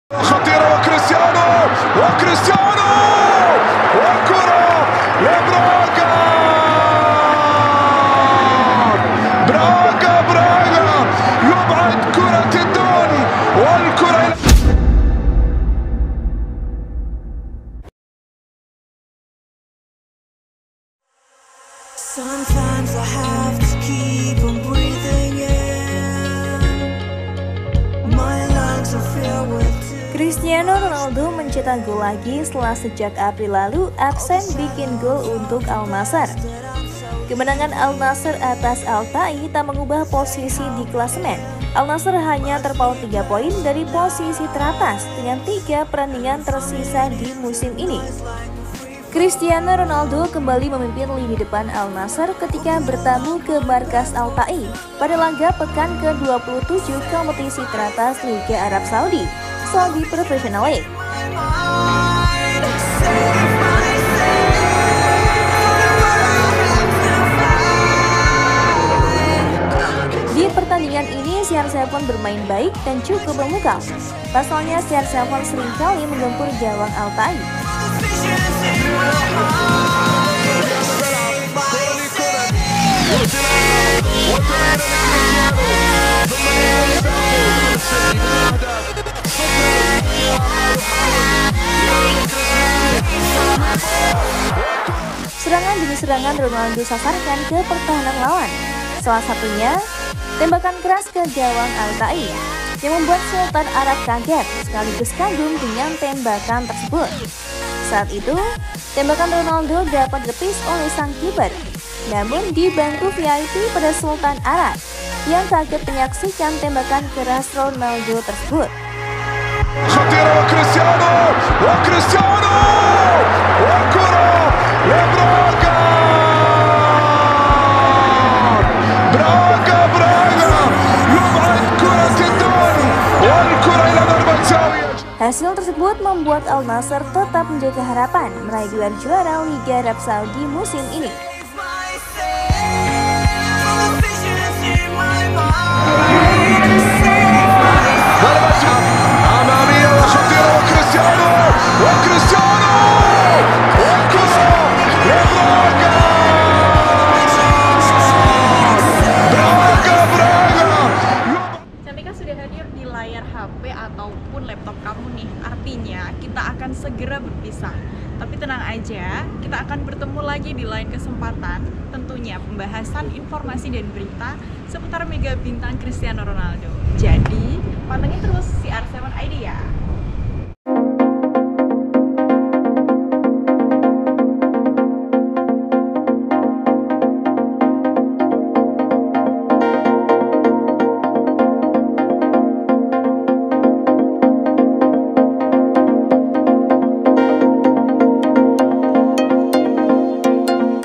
PARA SULTAN ARAB SAMPE TERKEJUT😱Reaksi Sultan Arab Saat Lihat Kerasnya Tendangan Ronaldo Vs Al Taee
PARA SULTAN ARAB SAMPE TERKEJUT 😱 Reaksi Sultan Arab Saat Melihat Kerasnya Tendangan Ronaldo Vs Al Taee

Cristiano Ronaldo cetak gol lagi setelah sejak April lalu absen buat gol untuk Al Nassr. Kemenangan Al Nassr atas Al Ta'ee tidak mengubah posisi di klasemen, meski jarak dengan Al Ittihad terpangkas jadi 3 poin. Dengan pemimpin liga, Al-Ittihad membuang keunggulan dua gol untuk bermain imbang 2-2 dengan Al Hilal, Al Nassr hanya terpaut tiga poin dari posisi teratas dengan tiga pertandingan tersisa di musim ini.

Saksikan informasi selengkapnya di dalam Video ini!